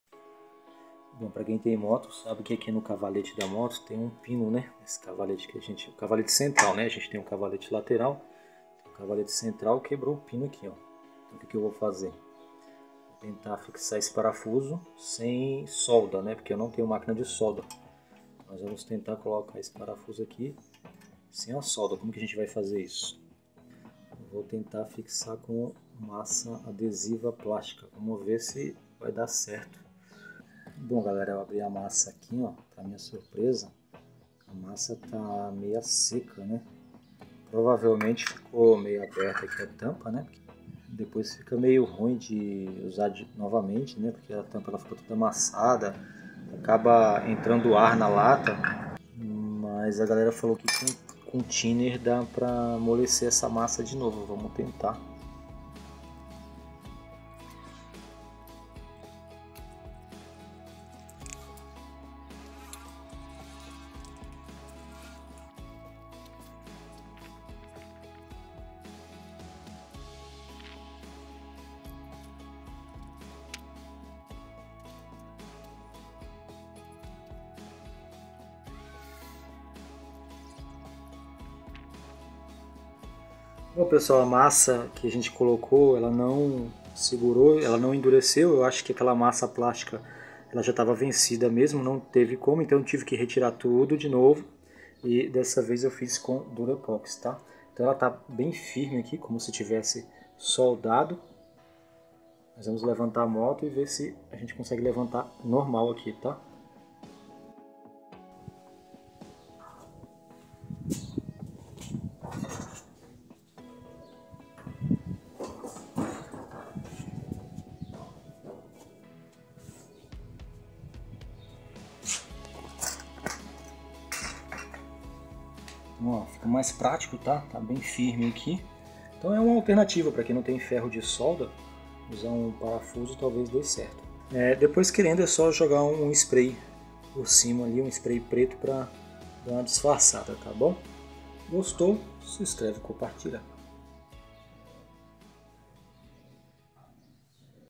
cavalete central cg titan fan quebrou
CAVALETE
CENTRAL

vídeo básico mostrando como recuperar cavalete central com pino quebrado sem maquina de solda